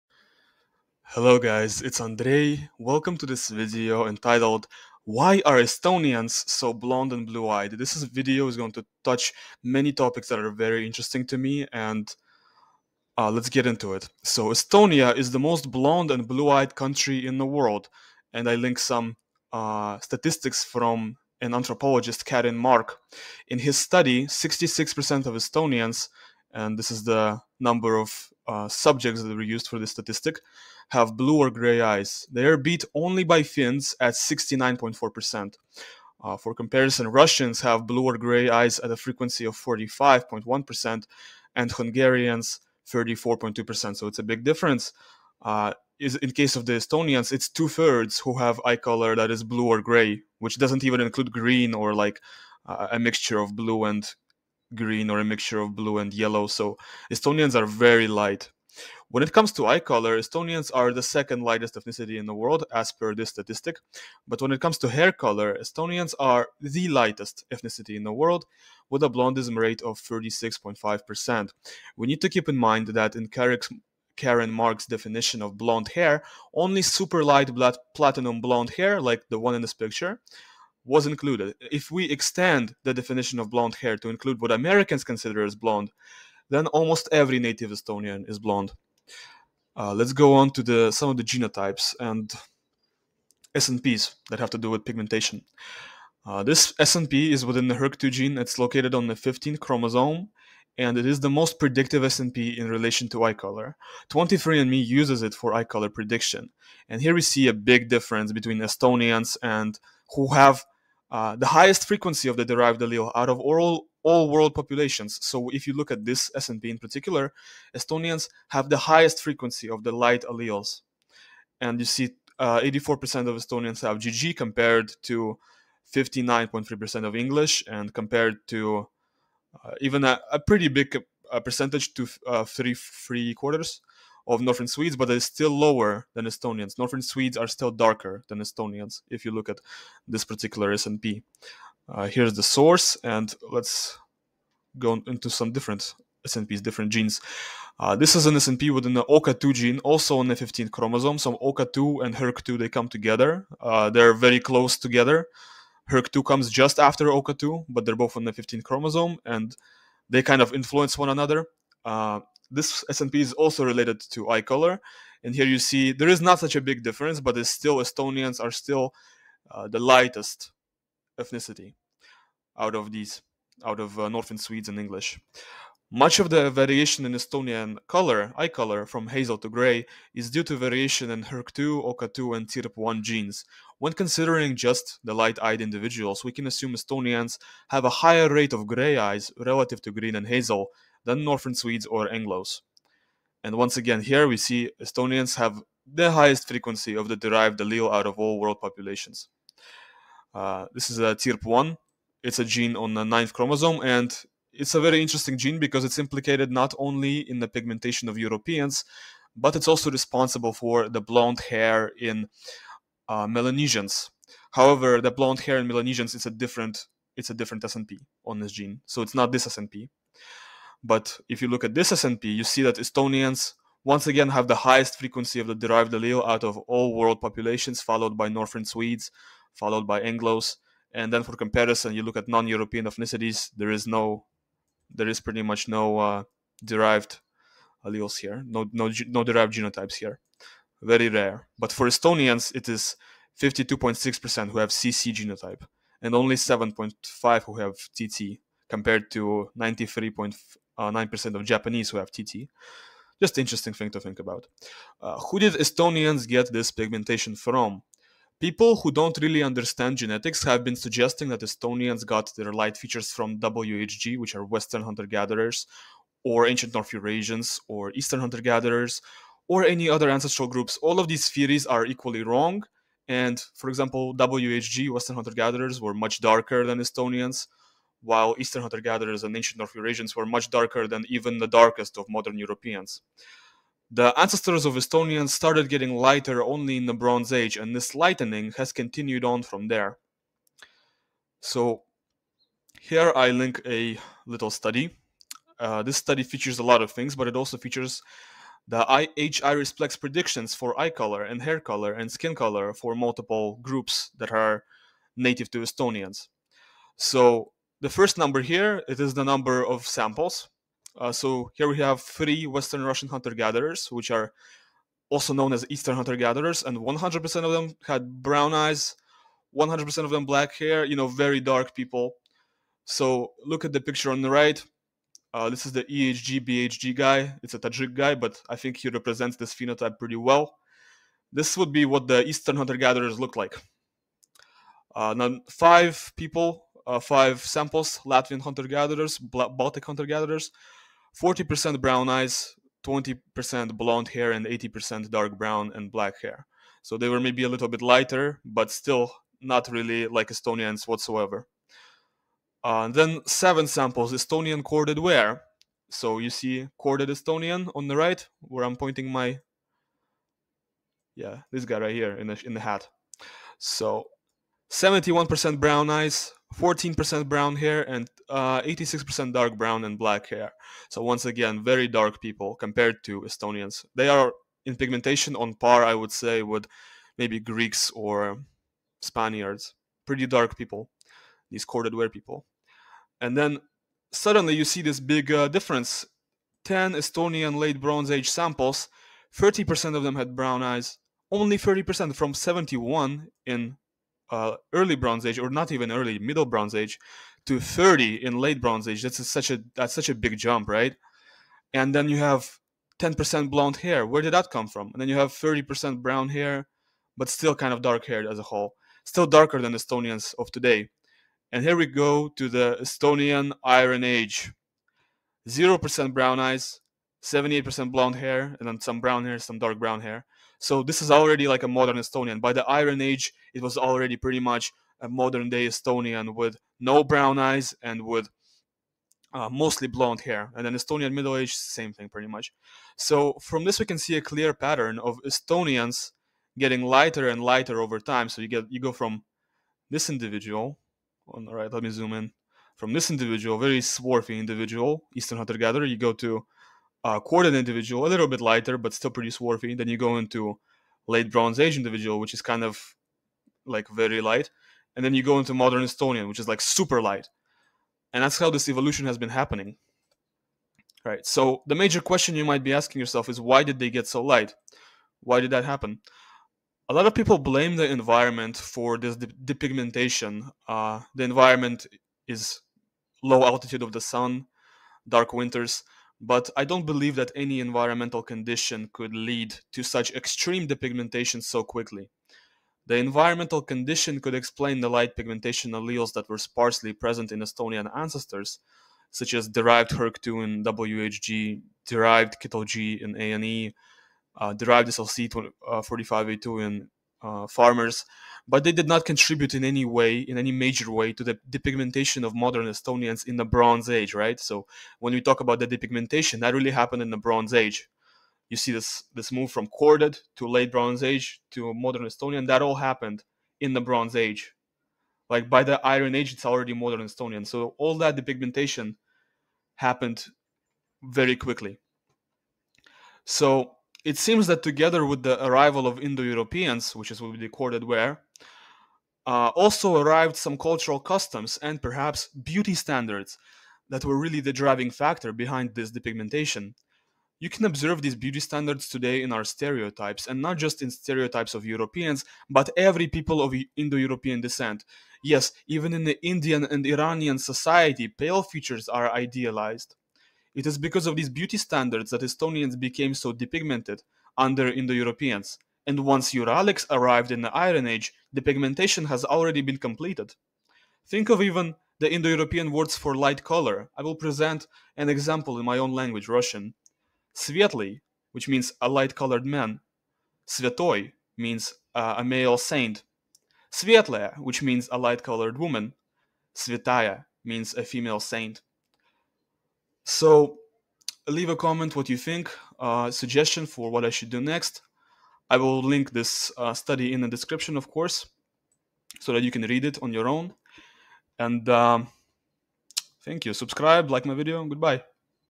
Estonians are the lightest people in the world. Why?
Study mentioned in video
A lot of people blame the environment for this depigmentation (low altitude of the sun, dark winters) but I don’t believe that any environmental condition could lead to such extreme depigmentation so quickly
The environment condition could explain the light pigmentation alleles that were sparsely present in Estonian ancestors (Derived HERC2 in WHG, derived KITLG in ANE, derived SLC45A2 in EEF) but they did not contribute in any major way to the depigmentation of modern Estonians in the Bronze Age.
  It seems that together with the arrival of Indo-Europeans, also arrived some cultural customs - and perhaps beauty standards, that were really the driving factor behind this depigmentation.
  You can observe these beauty standards today in our stereotypes, and not just in the stereotypes of Europeans but EVERY people of Indo-European descent. Yes, even in the Indian and Iranian society pale features are idealized.
  It is because of these beauty standards that Estonians become so depigmented under Indo-Europeans. And once Uralics arrived in the iron Age, depigmentation has already been completed.
   Think of even the Indo-European words for light color. I will present an example in my own language, Russian:
Svetly (Light colored male), Svyatoy (A male Saint), Svetlaya (Light colored woman), Svyataya (A female saint)